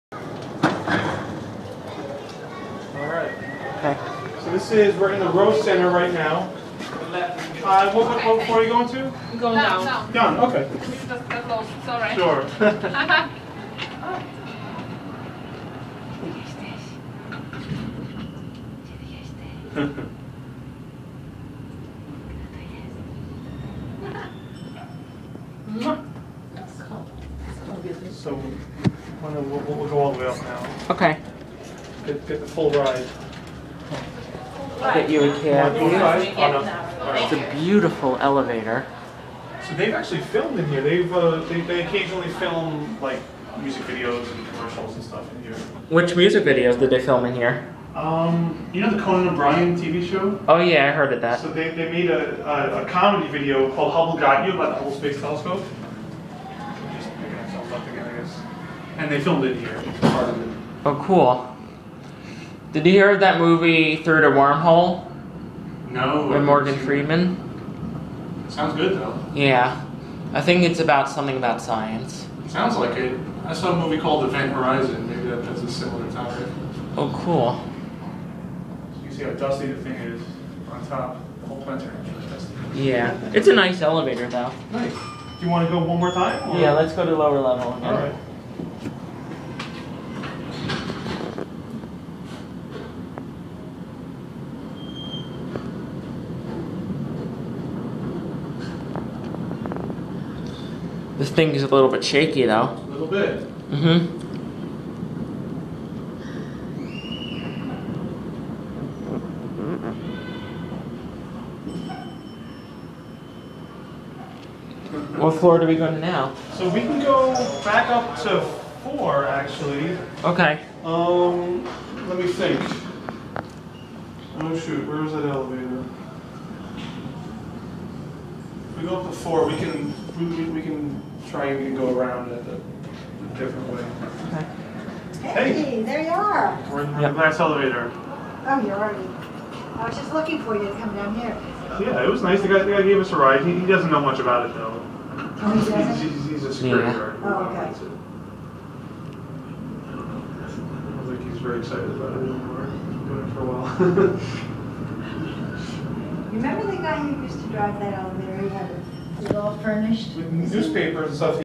?? Traction Scenic Elevators At The American Museum Of Natural History In NYC
These are the very nice ?? traction scenic elevators at the American Museum Of Natural History in NYC.